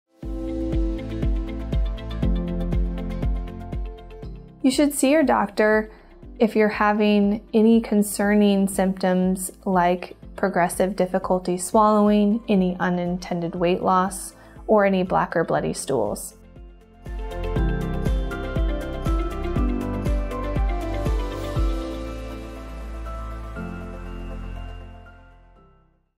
When should I see my doctor? | Esophageal Cancer Awareness Month
Join Jane Ohde, DO, general surgeon at Northern Nevada Medical Group as she discusses when you should see your doctor about esophageal-related symptoms.